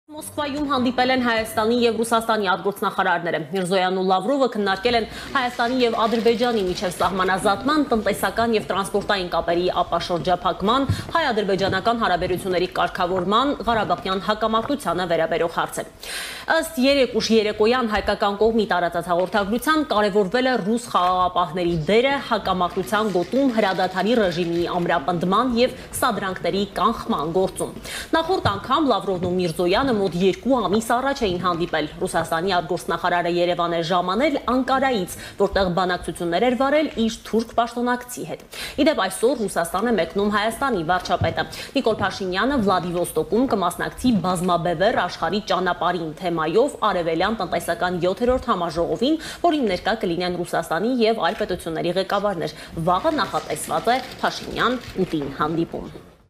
Միրզոյանը Լավրովի հետ է հանդիպել Մոսկվայում. Փաշինյանը մեկնում է Վլադիվոստոկ
Մոսկվայում հանդիպել են Հայաստանի և Ռուսաստանի արտգործնախարարները: Միրզոյանն ու Լավրովը քննարկել են Հայաստանի և Ադրբեջանի միջև սահմանազատման, տնտեսական և տրանսպորտային կապերի ապաշրջափակման, Հայ-ադրբեջանական հարաբերությունների կարգավորման, Ղարաբաղյան հակամարտությանը վերաբերող հարցեր: Ըստ հայկական կողմի պաշտոնական հաղորդագրության՝ կարևորվել է ռուս խաղաղապահների դերը՝ «հակամարտության գոտում հրադադարի ռեժիմի ամրապնդման և սադրանքների կանխման գործում»։